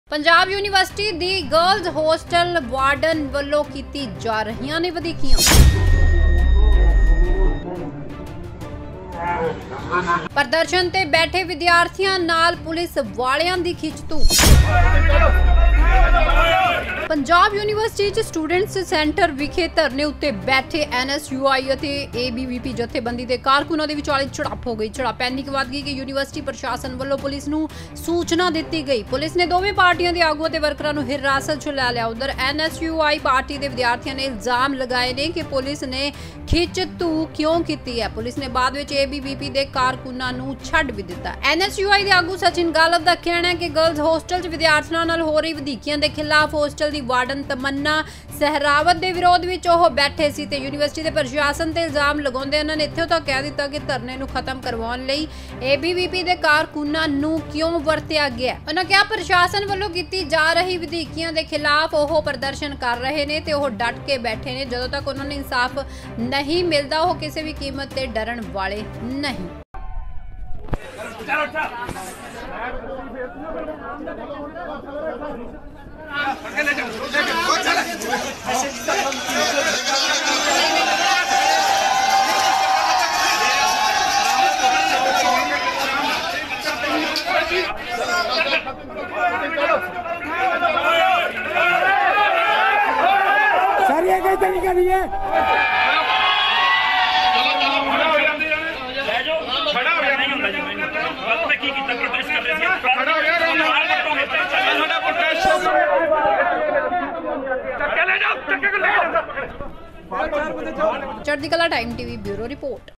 PU ਦੀਆਂ ਵਿਦਿਆਰਥਣਾ ਨਾਲ ਵਾਰਡਨ ਨੇ ਕੀਤਾ ਕੁਝ ਐਸਾ, ਪੈ ਗਿਆ ਗਾਹ, ਦੇਖੋ ਪੁਲਿਸ ਨਾਲ ਝੜਪਾਂ
- ਪੰਜਾਬ ਯੂਨੀਵਰਸਿਟੀ ਦੀਆਂ ਵਿਿਦਆਰਥਣਾਂ ਨਾਲ ਵਾਰਡਨ ਨੇ ਕੀਤਾ ਕੁਝ ਐਸਾ
- ਕੁੜੀਆਂ ਲਈ ਆਵਾਜ਼ ਚੁੱਕਣ ਆਏ NSUI ਦੇ ਵਿਿਦਆਰਥੀਆਂ ਨਾਲ ਪੁਲਿਸ ਦੀ ਖਿੱਚਧੂਹ